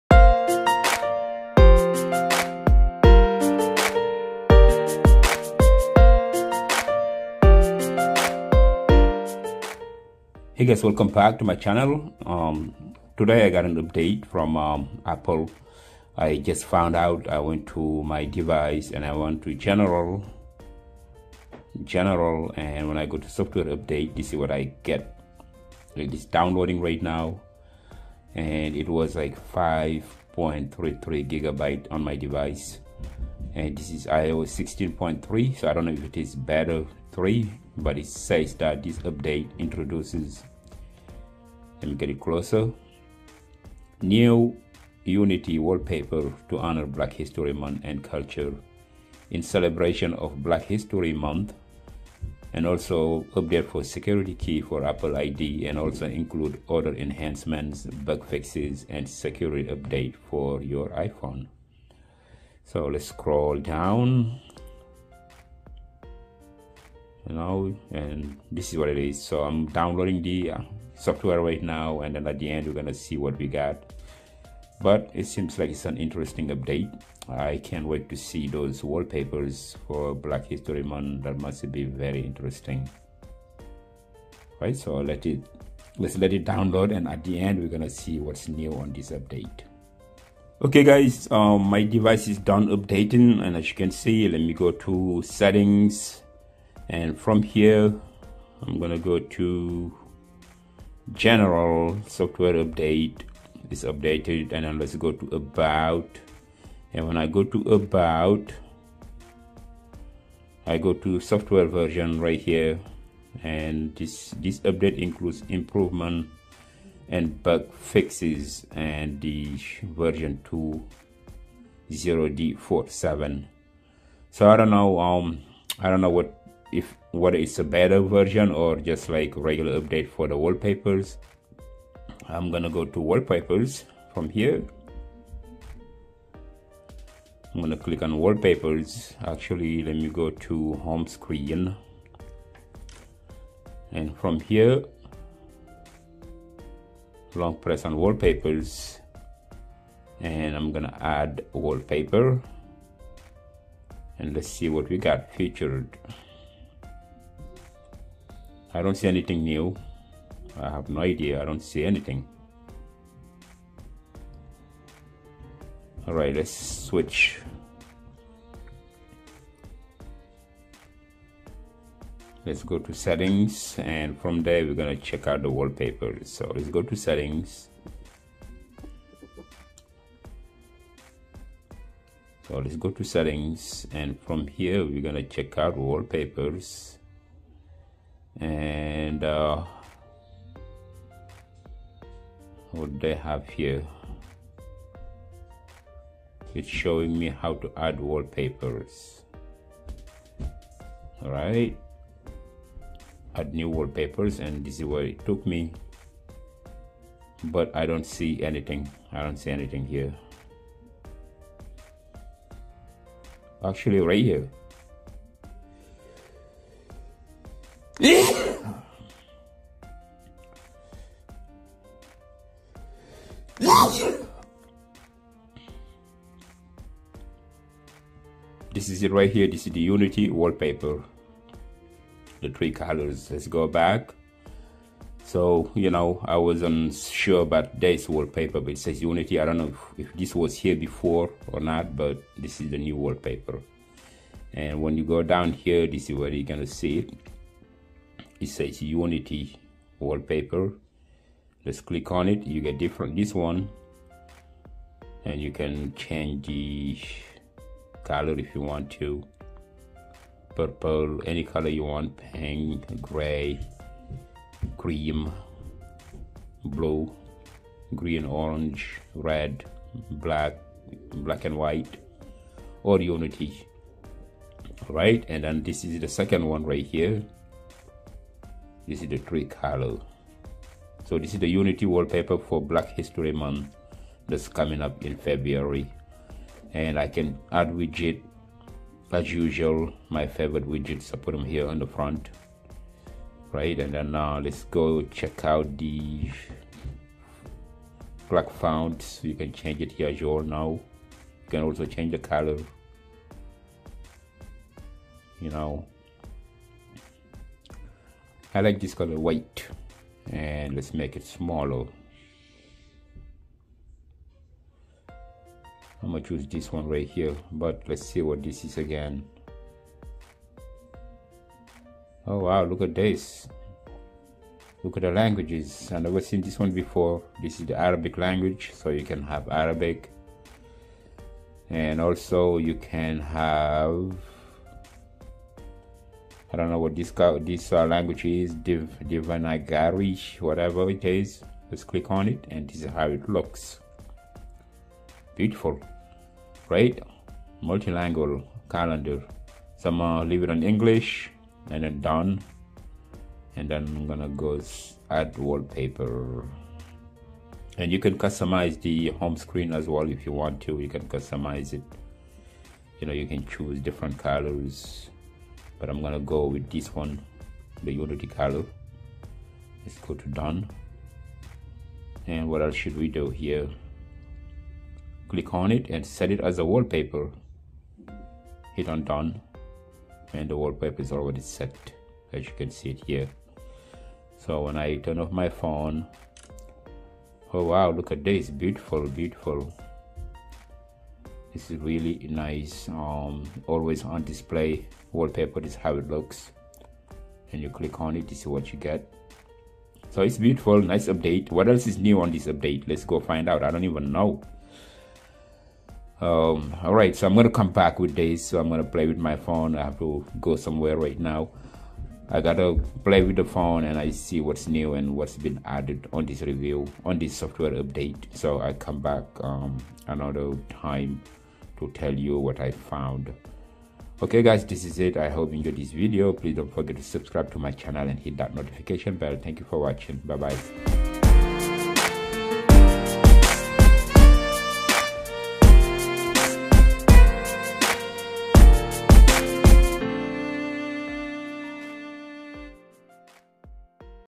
iOS 16.3 RC, New Unity wallpaper, what else is new
New iOS update,  iOS 16.3 RC with black history month unity wallpaper, security key, SOS security update,  and more